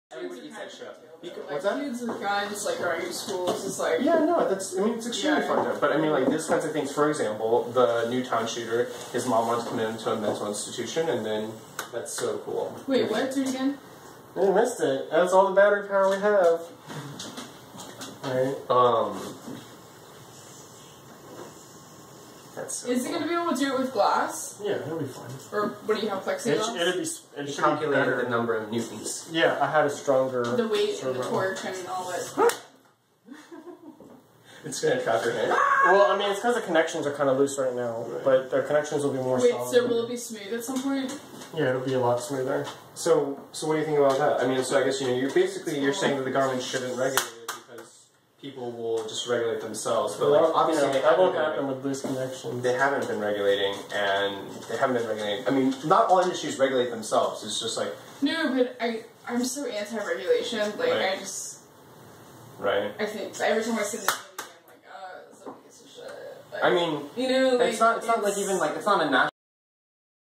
Servo Side Panel
Using arduino and servo to open computer side panel on hinges. Cost ~$50 in parts.